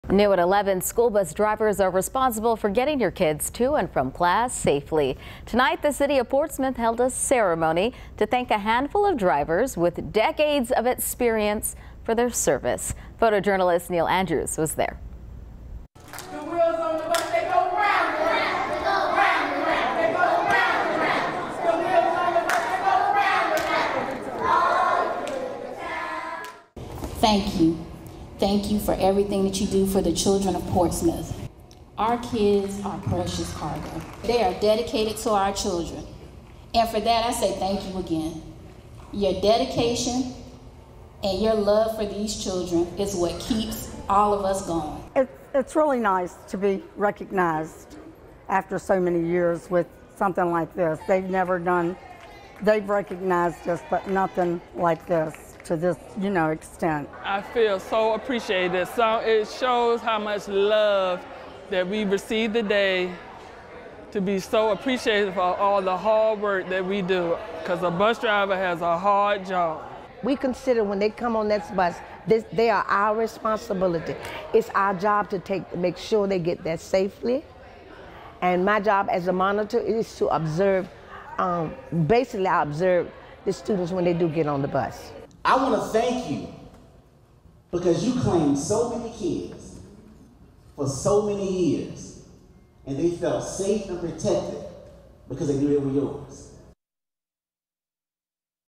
Honoring school bus drivers in Portsmouth
School bus drivers are responsible for getting your kids to and from class safely! On Wednesday night, the City of Portsmouth held a ceremony to thank a handful of drivers with decades of experience for their service. Photojournalist Neal Andrews was there.